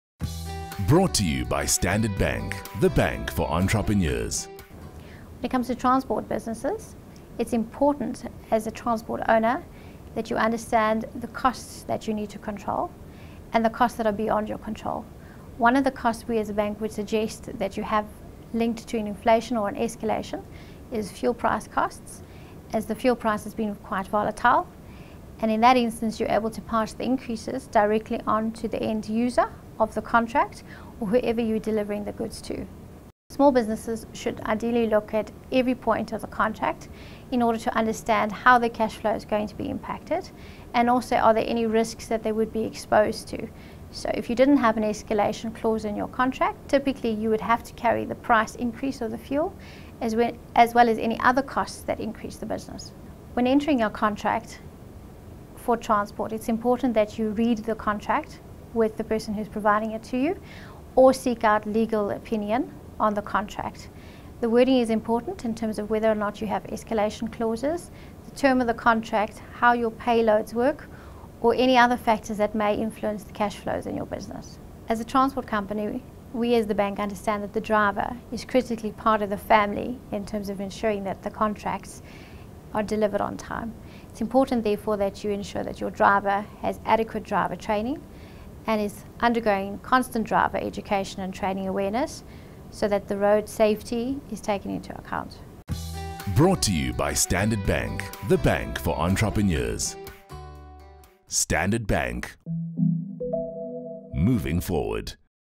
Controlling costs & risks in your transport business
Controlling costs is an important factor in the transport & logistics industry. Toni Fritz, Head of Vehicle & Asset Finance, Business, discusses key considerations for small businesses to plan for sudden costs.